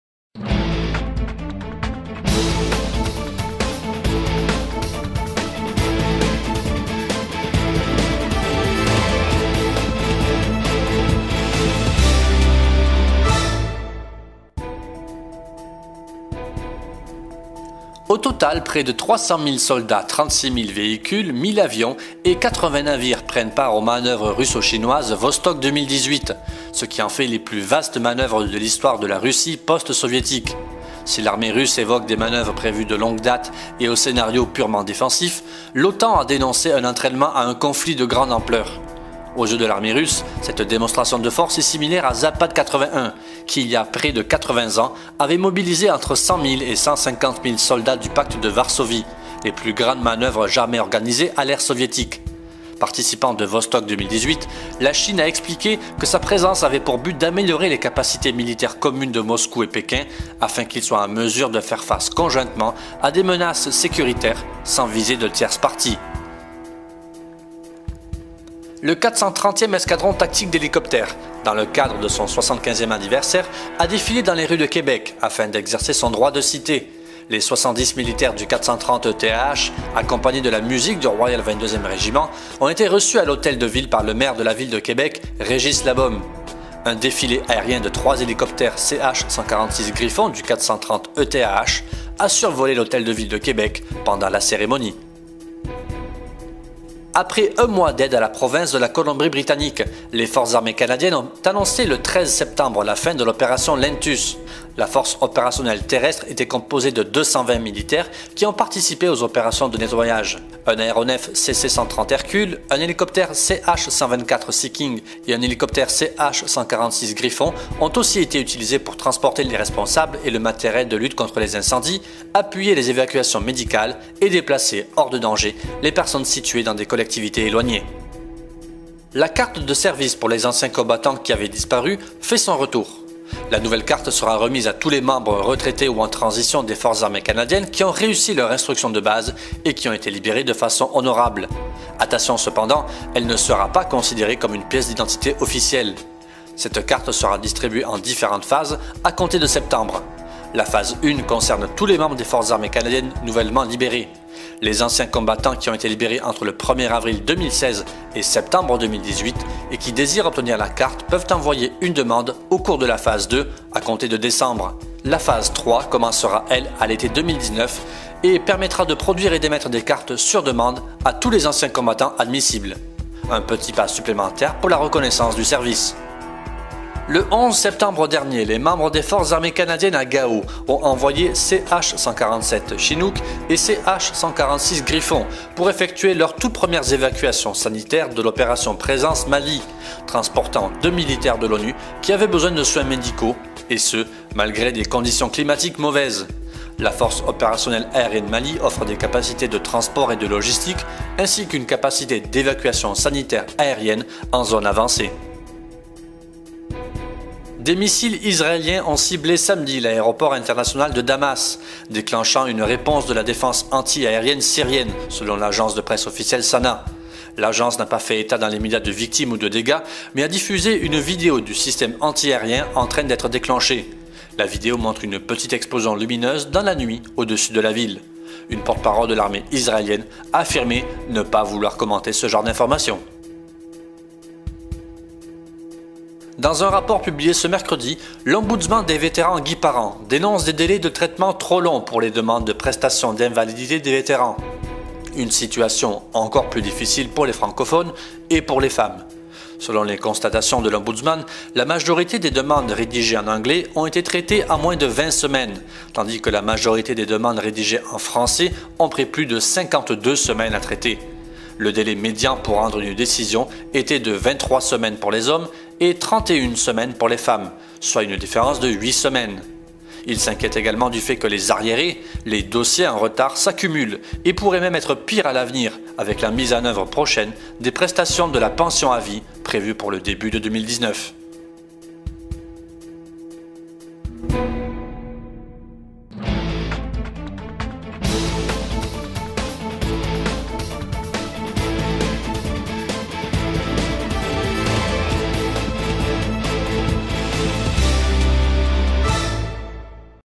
REVUE DU 16/09/2018 DE 45ENORD.CA
Les sujets de la revue de la semaine du 10 au 16 septembre 2018
- exercice Vostok 2018 majeur en Russie
- droit de cité du 430e escadron tactique d'hélicoptères
- fin de l'Opération LENTUS en Colombie-Britannique
- nouvelle carte de service pour les anciens combattants
- premières évacuations sanitaires pour l'Opération PRESENCE
- missiles israéliens interceptés
- rapport accablant pour les délais des prestations d'invalidité